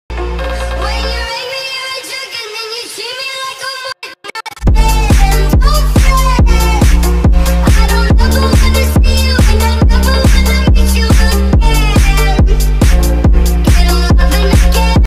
Hammer-Ausgleich von Celina Degen | Österreich vs. Belgien | Frauen-Nationalteam
Frauen-Nationalteam - Freundschaftsspiel am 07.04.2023. Torclip von Celina Degen zum 2:2 gegen Belgien.